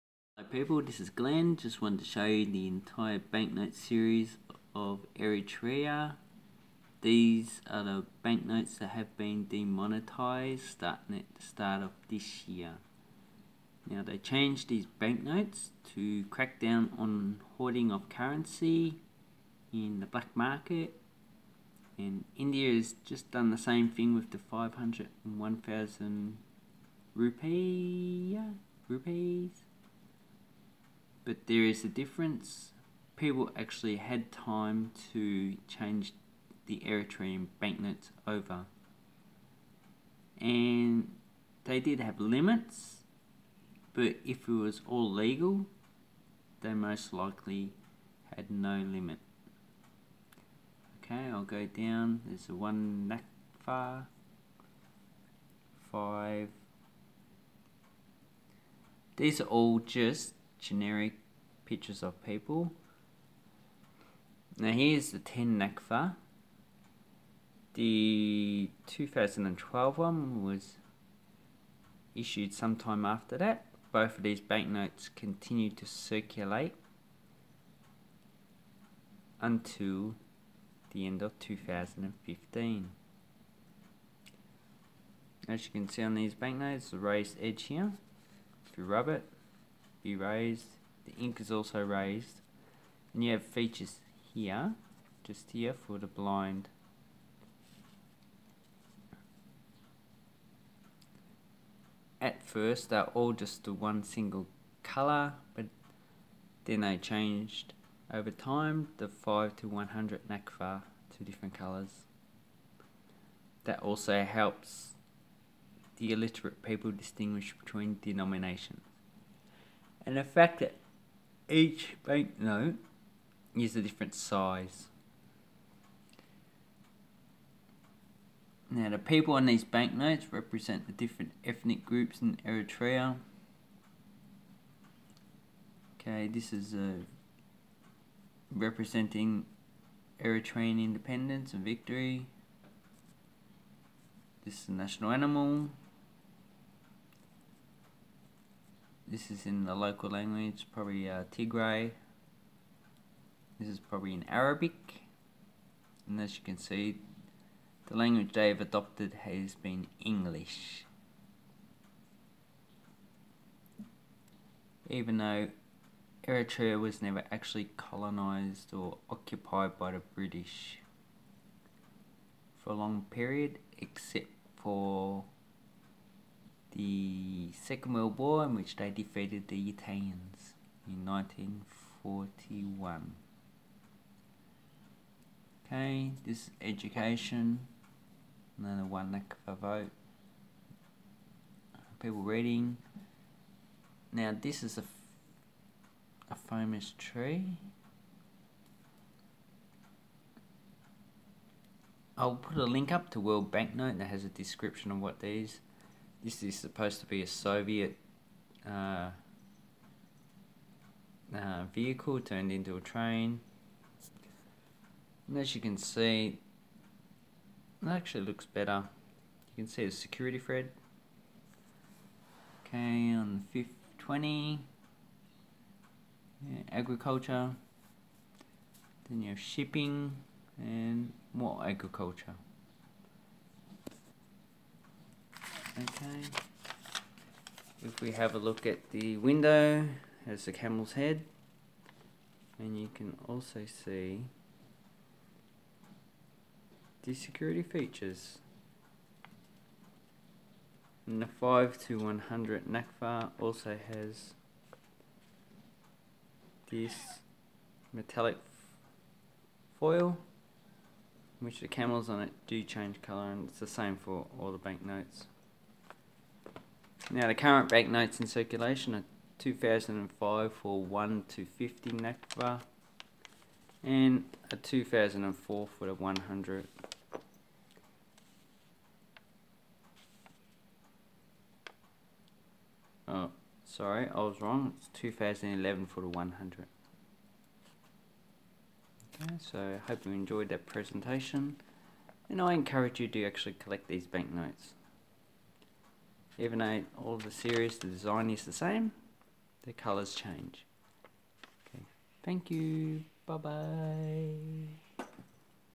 Eritrean banknotes
Demonetised  banknotes. Current circulating banknotes are 2015 for 1-50 nakfa and 2011 for 100 nakfa.
To buy these banknotes:
1, 5, 10 nakfa

10 & 20 nakfa

50 nakfa

100 nakfa